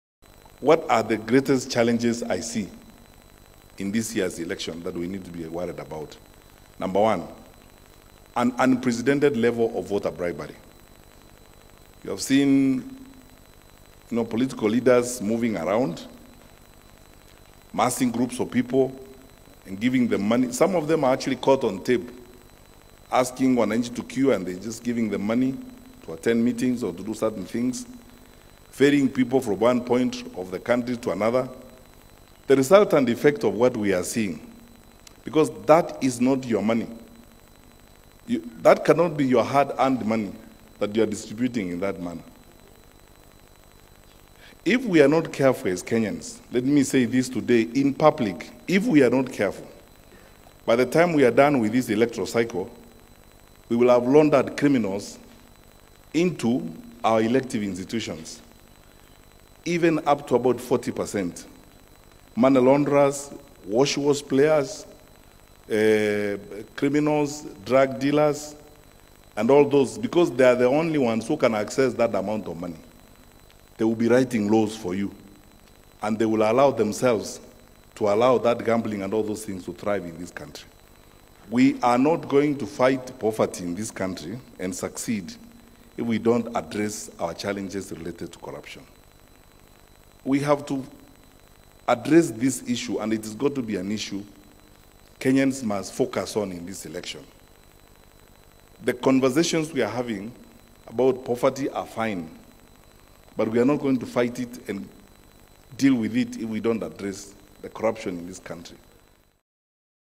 "...IF WE ARE NOT CAREFUL.." CS MATIANG'I SENDS OUT A WARNING TO KENYANS.
Daily Video Updates.